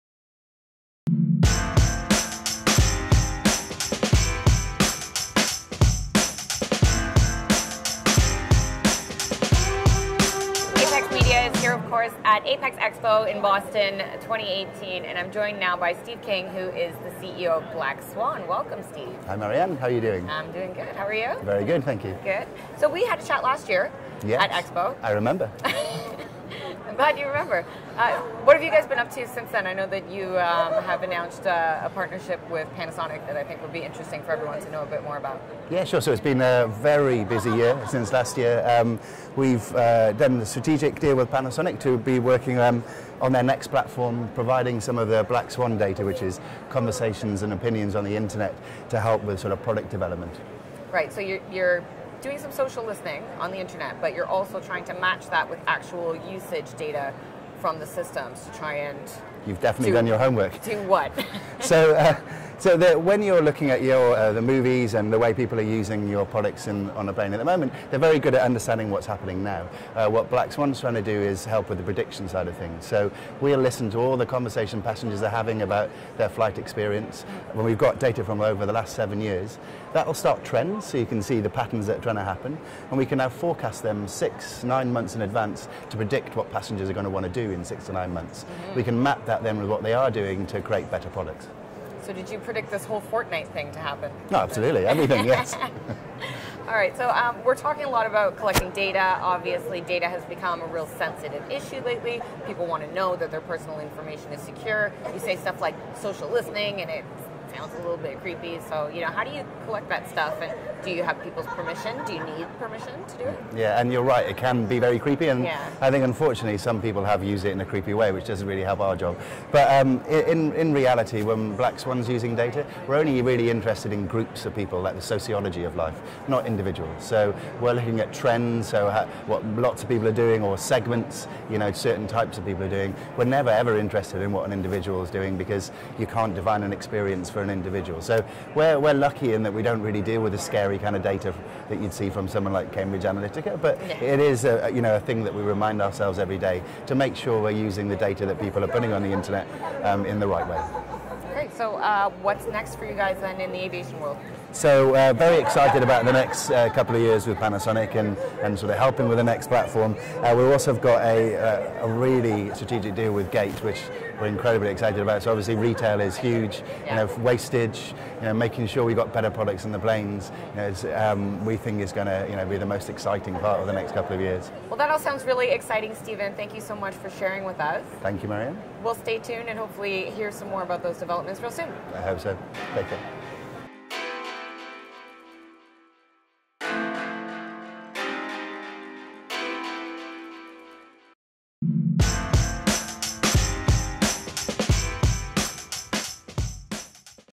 APEX @ EXPO 2018 - Black Swan on the Power of Social Listening
APEX is about

Steve King, CEO of Black Swan Data, explains how the company's new strategic partnership will help Panasonic Avionics when it comes to new product development.

APEX@ is made possible by the support of West Entertainment.